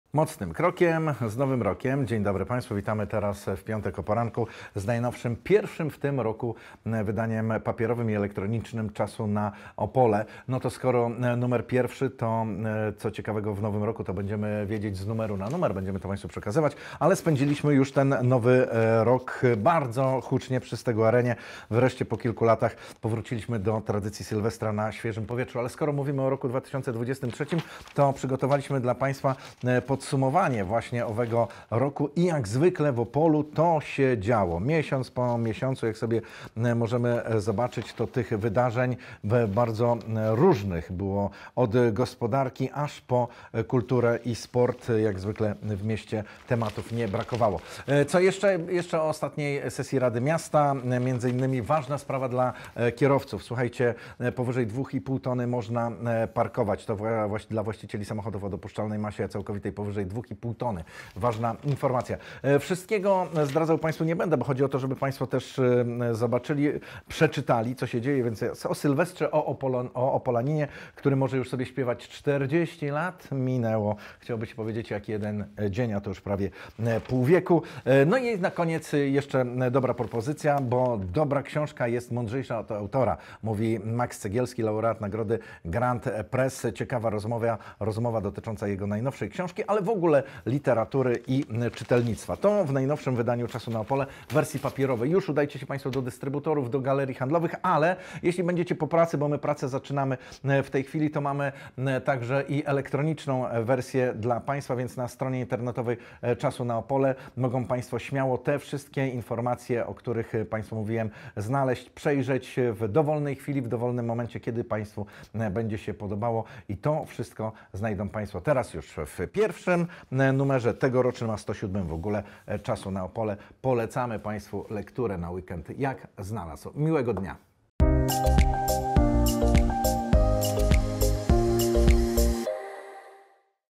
Noworoczny numer Czasu na Opole już dostępny!
Rozpoczynamy kolejny rok z naszym bezpłatnym dwutygodnikiem, który znajdą Państwo w wielu miejscach Opola (ekspozytory w galeriach handlowych oraz miejskich instytucjach) a także w wersji elektronicznej. Co przygotowaliśmy w pierwszym numerze? Zobaczcie Państwo.

📰Szukasz więcej informacji na temat E-GAZETA? Sprawdź